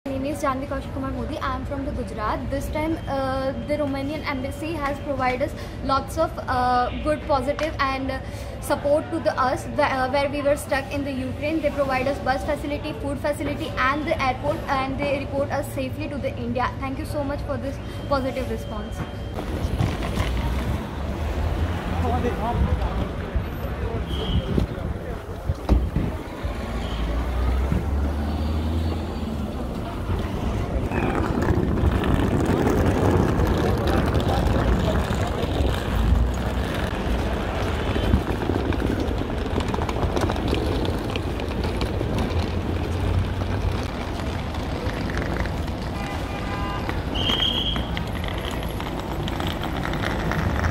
রোমানিয়া সরকারের সাহায্যে যুদ্ধ বিধ্বস্ত ইউক্রেন থেকে ভারতীয় পড়ুয়াদের দেশে ফেরানোর কাজ চলছে
Russia-Ukraine War: সরকারের সাহায্যে যুদ্ধ বিধ্বস্ত ইউক্রেন থেকে ভারতীয় পড়ুয়াদের দেশে ফেরানোর কাজ চলছে